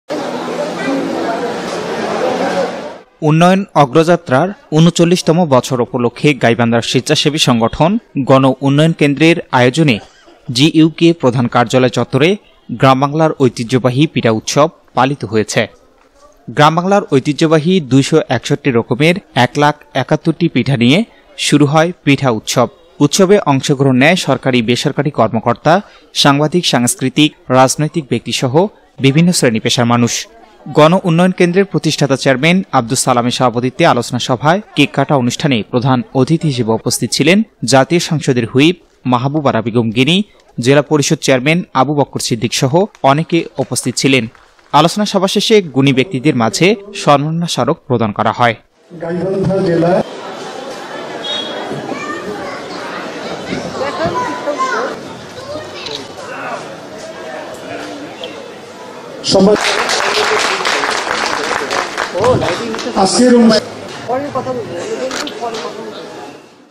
গাইবান্ধায় গ্রামবাংলার ঐতিহ্যবাহি পিঠা উৎসব অনুষ্ঠিত || ABN 24 NEWS
উন্নয়ন অগ্রযাত্রার ৩৯ তম বছর উপলক্ষে গাইবান্ধার সেস্বাসেবী সংগঠন গণ উন্নয়ন কেন্দ্রের আয়োজনে জিইউকে প্রধান কার্যালয় চত্তরে গ্রাম বাংলার ঐতিহ্যবাহী পিঠা উৎসব পালিত হয়েছে।